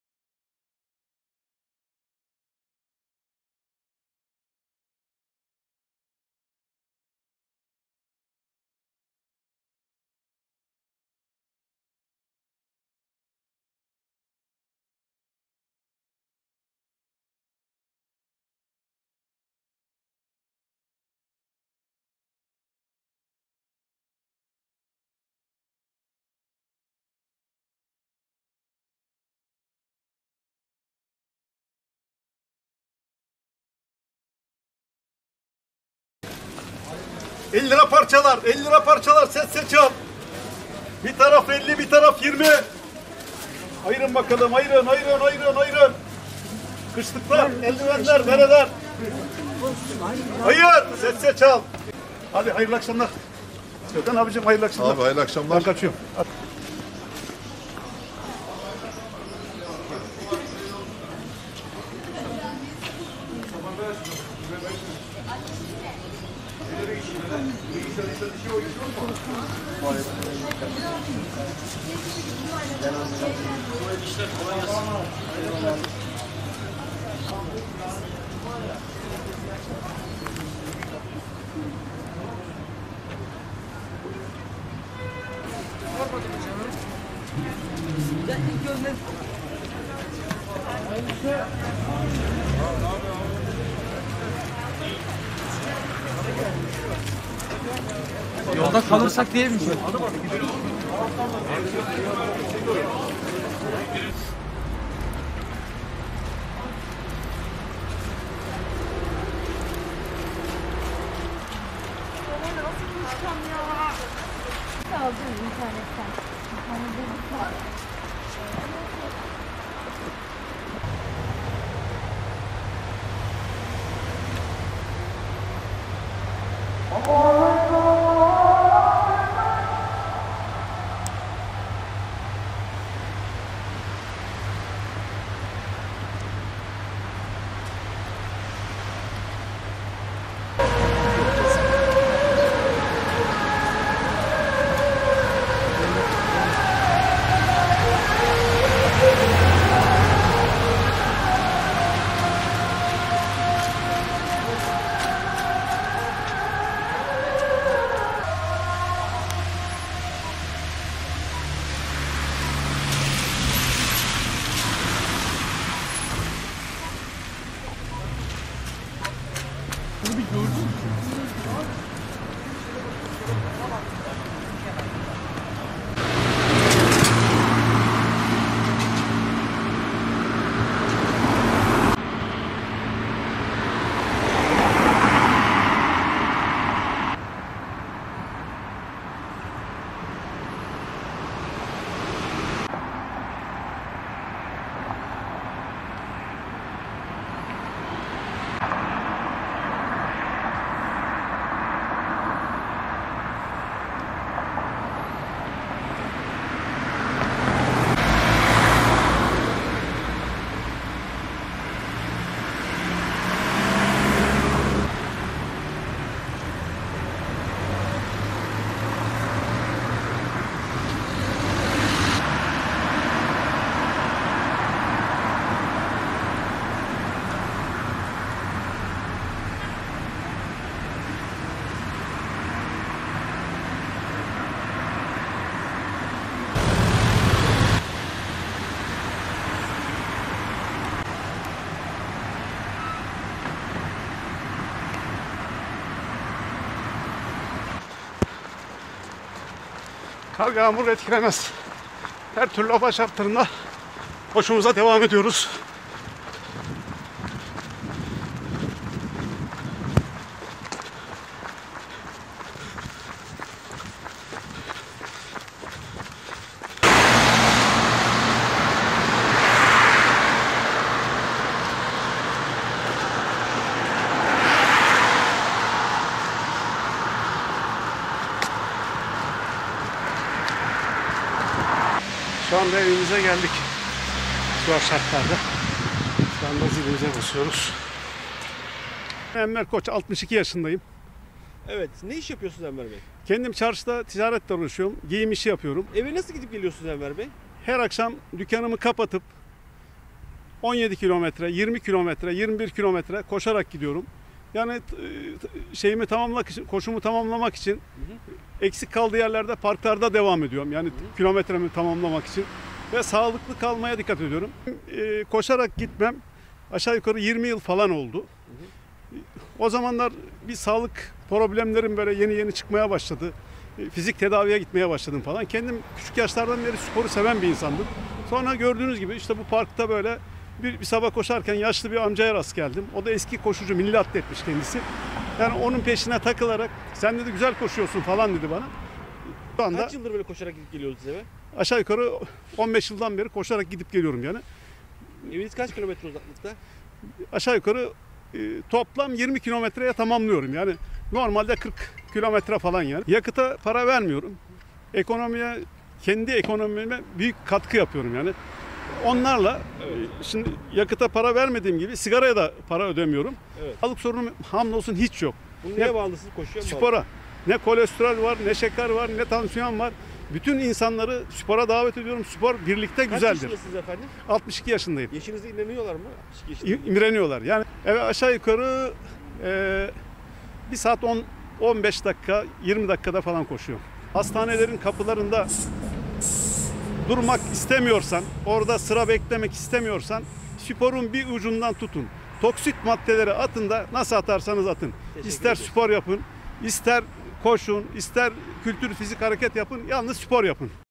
Kar kış dinlemiyor! Her gün işten evine koşarak gidiyor
Bursa'da atletizm de yapan bir esnaf; her gün 20 kilometre uzaklıktaki evine koşarak gidiyor. Enver Koç, hem sağlıklı kalıyor hem de ulaşıma ücret ödemiyor.

Kapalıçarşı'da esnaflık yapan Enver Koç, (62) 40 yaşında sağlık sorunları yaşayınca sigarayı bırakıp spor yapmaya başladı. Atletizme merak salan Koç, ulusal ve uluslararası maratonlara katılmanın yanı sıra her gün işinden evine koşarak gidip gelmeye başladı.

Spor yapmaya başladıktan sonra tüm sağlık sorunlarından kurtulan Enver Koç, şimdi 20 kilometre olan evinden işine her gün koşarak gidip geliyor. Bu sayede hem sağlıklı kalıyor hem de hiç bir ulaşım ücreti ödemeyerek tasarruf ediyor. Yaptığı spor sayesinde genç görünüme kavuşan Koç'un hiç bir sağlık sorunu bulunmuyor. Görenler ise 62 yaşında olduğuna inanmıyor.
Herkesi spor yapmaya davet eden Enver Koç, "Ben 40 yaşına kadar sigara içiyordum. Sağlık sorunları yaşayınca sigarayı bırakıp koşulara başladım. Spora başladıktan 3 yıl sonra evimden işime her gün koşarak gidip gelmeye başladım. 17 yıldır işten evime koşarak gidip geliyorum. Şimdi hiç bir sağlık sorunum yok, hiç bir şeyi perhiz etmiyorum. Yaptığım spor sayesinde genç bir görünüme kavuştum. Kimse 62 yaşında olduğuma inanmıyor. Ben çarşıda esnafım, evimle dükkanım arası 20 kilometre. Her gün evden işe koşarak gidip gelerek sağlıklı kalıyorum. Ayrıca ulaşıma hiç ücret ödemiyorum. Spor yapın sağlıklı kalın" dedi.